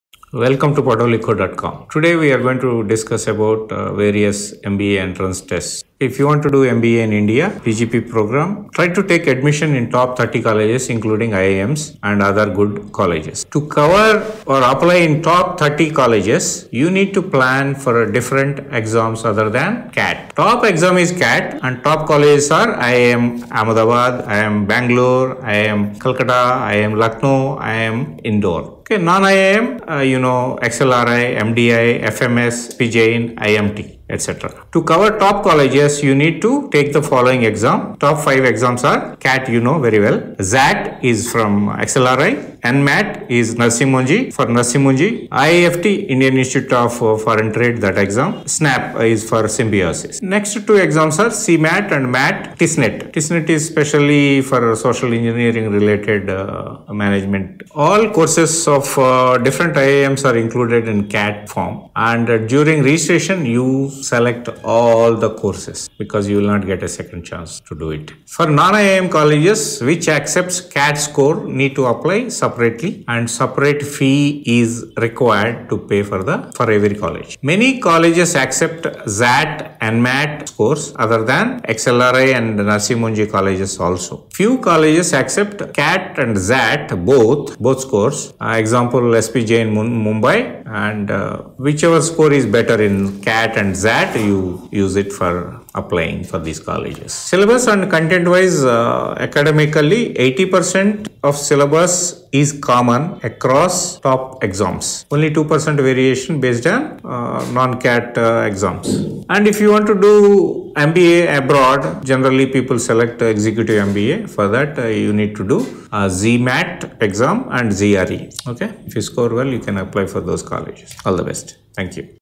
TOP 5 MBA EXAM
Based on various parameters of college performance, the All India ranking of MBA colleges is defined.
For better uplift in career, ROI, Brand, Campus Placement, etc., Top 30 All India MBA Colleges are good. Any student, who wants to take admission to MBA college, the first choice is, of course, IIM's but to increase the horizon of opportunity, it will be better to apply few other exams also. Based on the performance of these exams, the option for good ranked non-IIM Colleges will be opened.
This Session will be discussed about the Top 5 MBA Exams, which includes CAT, XAT, NMAT, IIFT & SNAP and Based on the performance in these exams, almost all top 30 colleges can be applied.

Address: 4th Cross, 1st Main Rd, Talacauvery Layout, Basavanagara, Bengaluru, Karnataka 560037